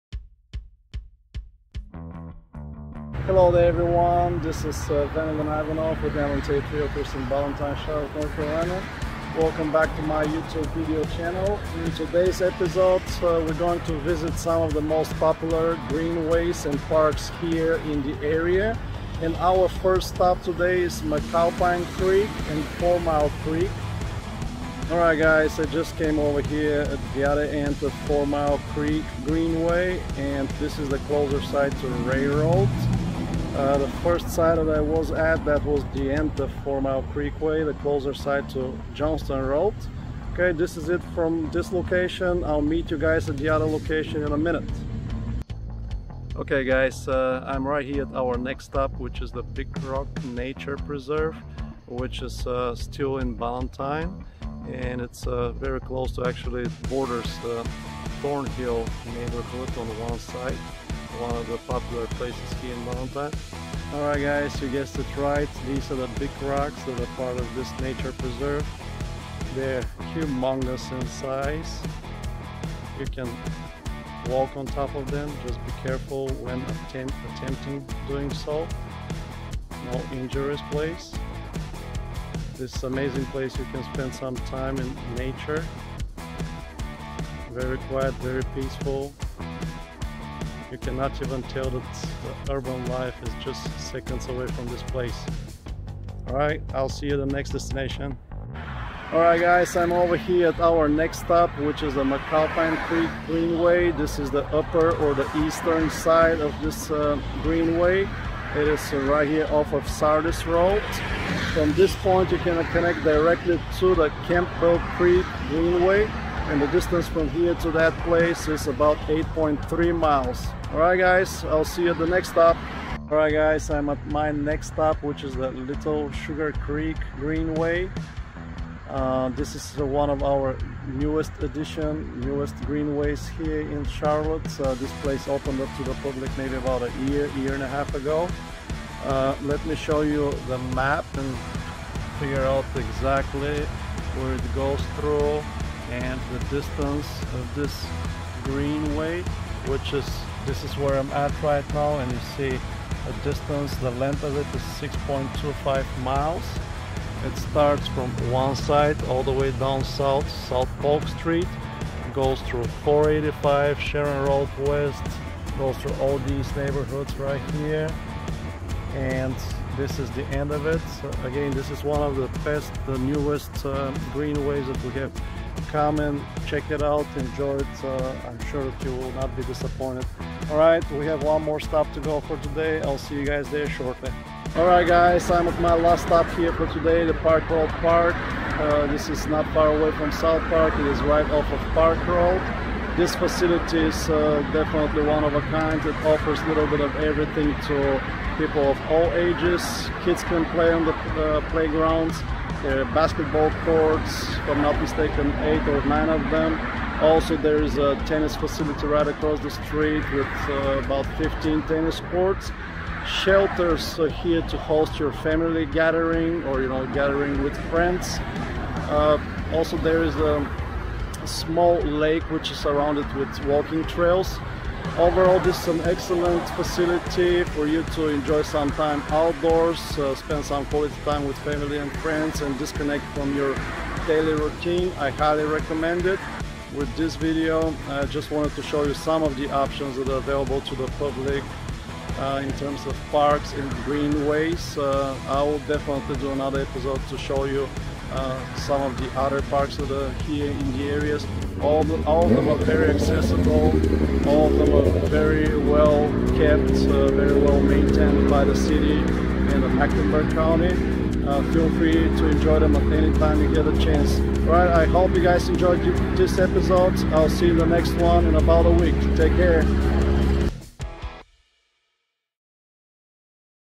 Greenways and Parks in Charlotte, NC
Take a quick look at some of the parks and greenways that are available to the residents of Charlotte and their guest.

Park Road Park-
Features:
    72 acres
    12 lighted tennis courts
    8 basketball courts
    2 horseshoe pits
    2 concession stands
    6 baseball/softball fields
    Volleyball court
    4 picnic shelters
    Walking trails
    11 acre lake with pier

Upper McAlpine Creek Greenway-
Length:
Paved Greenway = 4.76 Miles
Cottonwood Nature Trail = 1.5 Miles
McAlpine Creek Cross County Course = 3.1 Miles (5K)

Big Rock Nature Preserve-
Big Rock Nature Preserve is named for the granite extrusions that lie at the heart of the preserve.

Lower McAlpine Creek/McMullen Creek/Four Mile Creek Greenways-
Length:
Paved, Gravel, and Boardwalk Greenway = 5.8 Miles

All of the listed parks and greenways are within 5 miles or less apart from each other. This is another proof of their accessibility to the residents of Mecklenburg County and the efforts the City of Charlotte and the Meck County are putting into making the area more bicycle friendly.